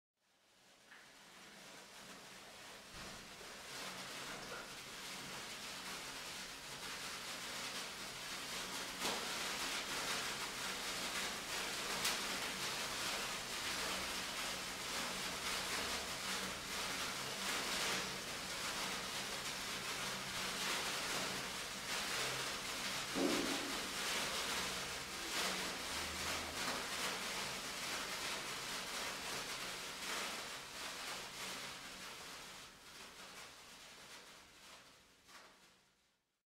Sparklers
One of the customs during Christmas and New Year is the lighting of sparklers. Children especially have great fun doing it. There's no data available on when this custom became popular but it's been definitely widespread since the second half of the 20th century.
Sound: Sparkler burning.